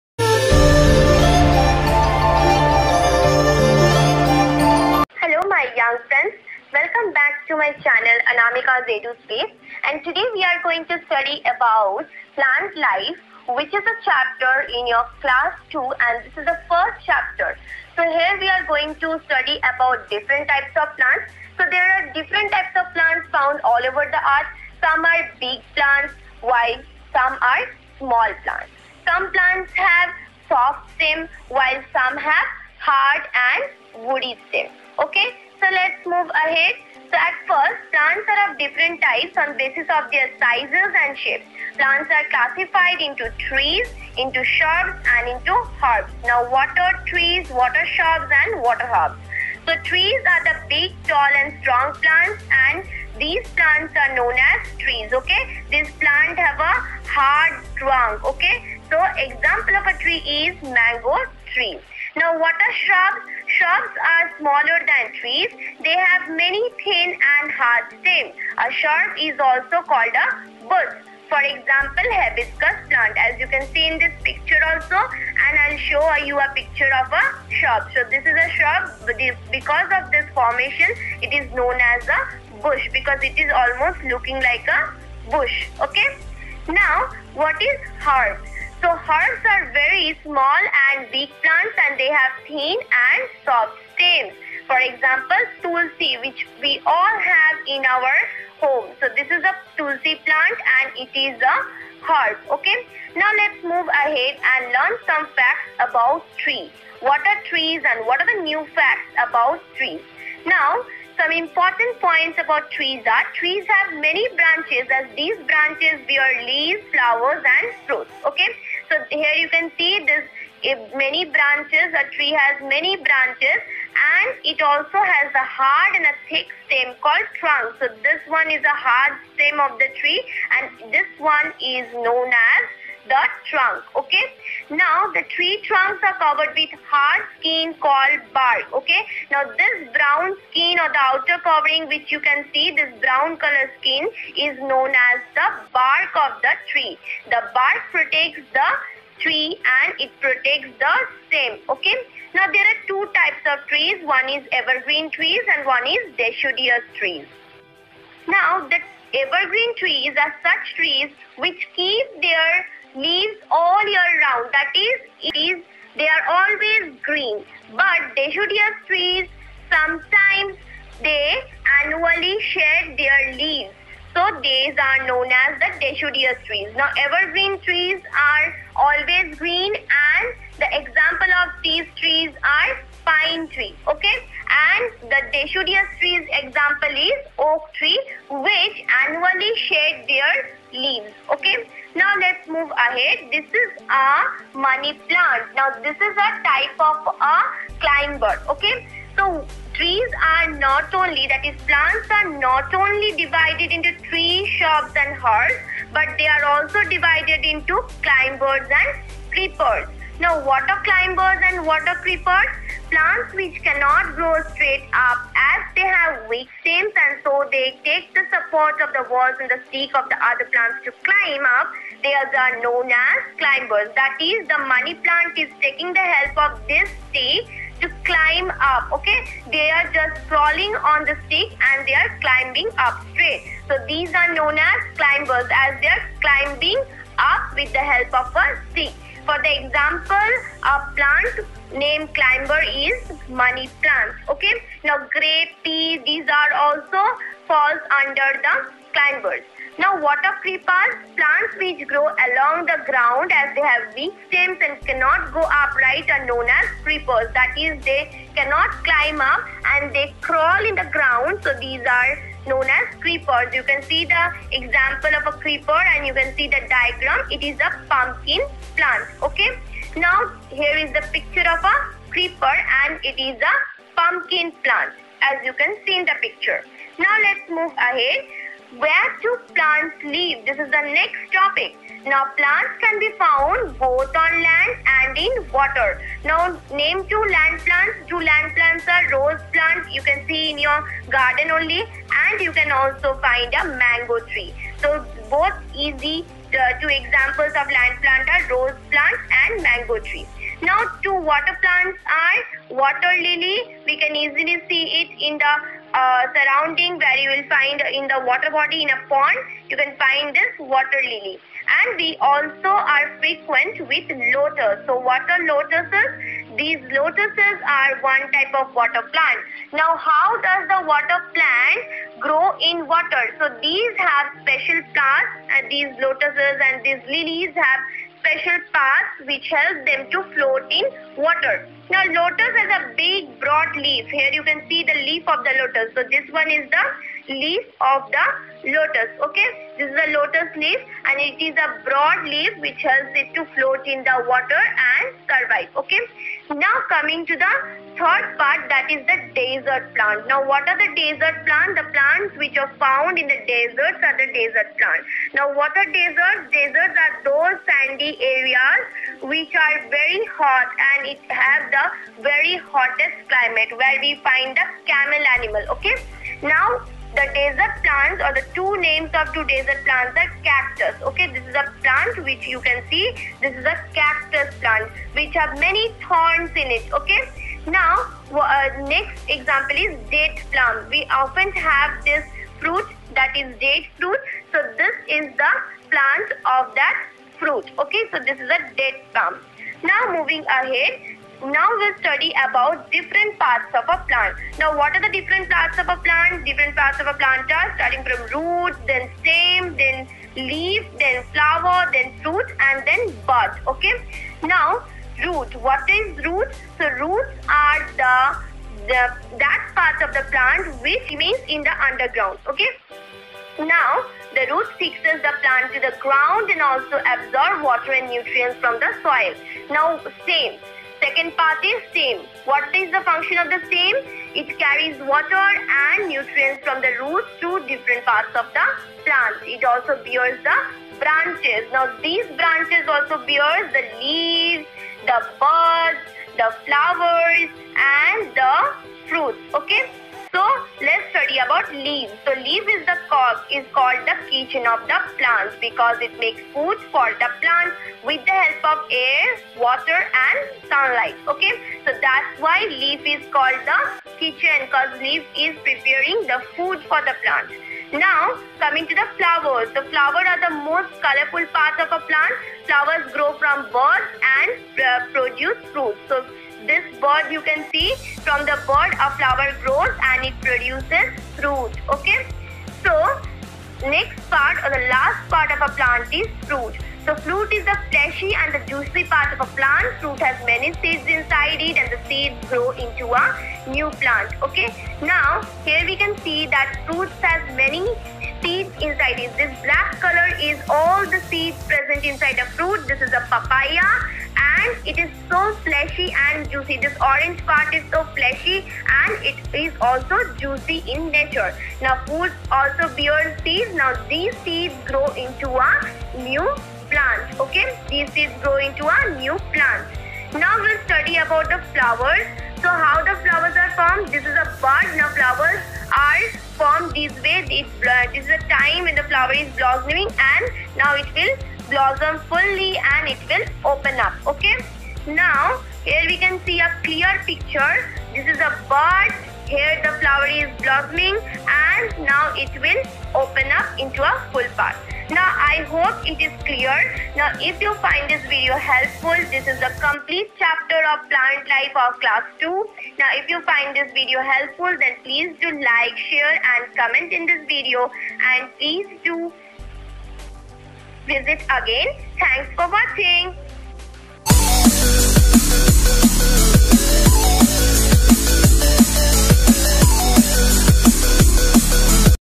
Plant Life, Class-2, Chapter-1•||©Anamika's Eduspace
This video contains the complete discussion about the first chapter i.e Plant Life present in your science text book.

If you like this video, then please do 🤗